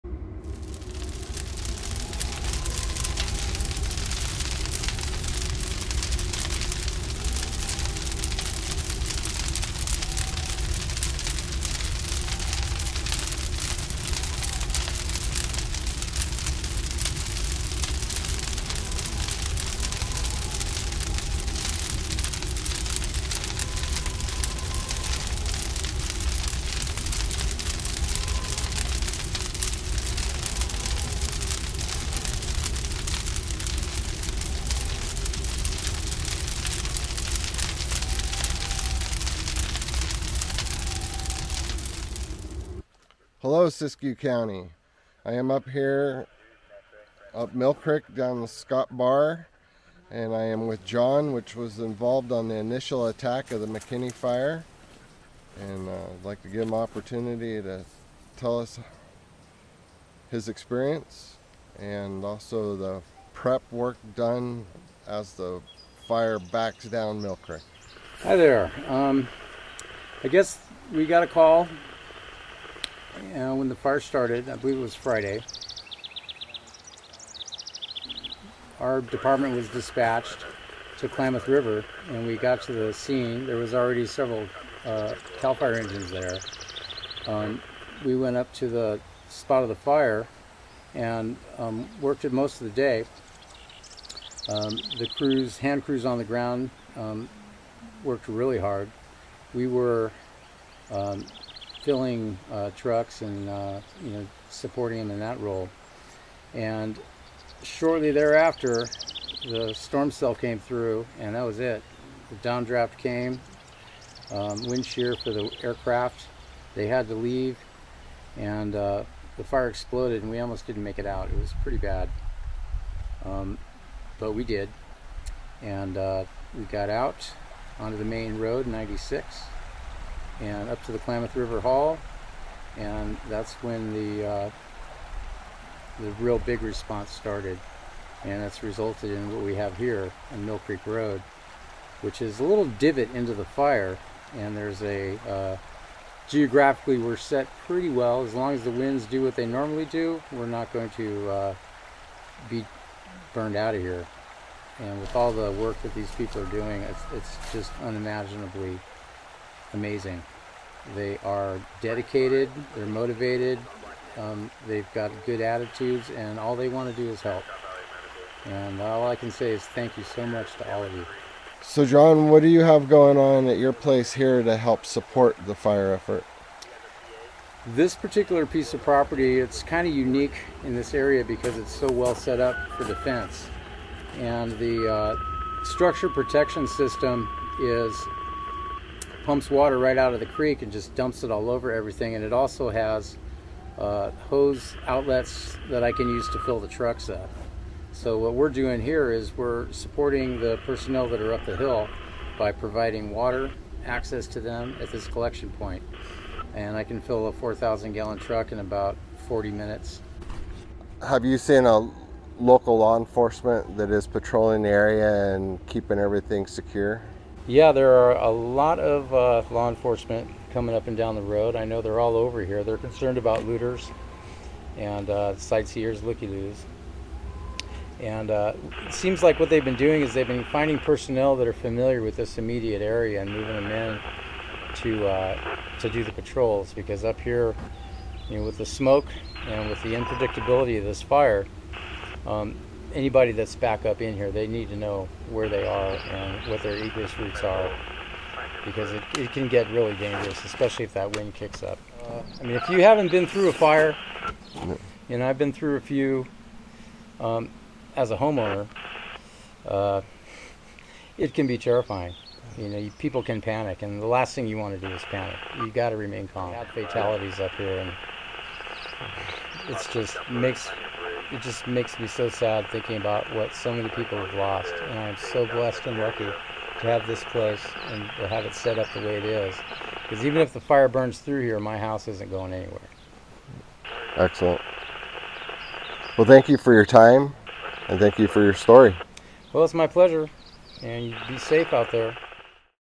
McKinney Beyond Words HD 720p
Interview with Jon whom took this video while responding to the fire with another engine. This video was taken around 9 pm between Walker Rd. and the Klamath River Hall, Just before it jumped the line.

I meet Jon while taking pictures of the dozer line and hand crews working up Mill Creek Rd in Scott Bar, California

His happiness for life and commitment to the community is inspiring.
Siskiyou strong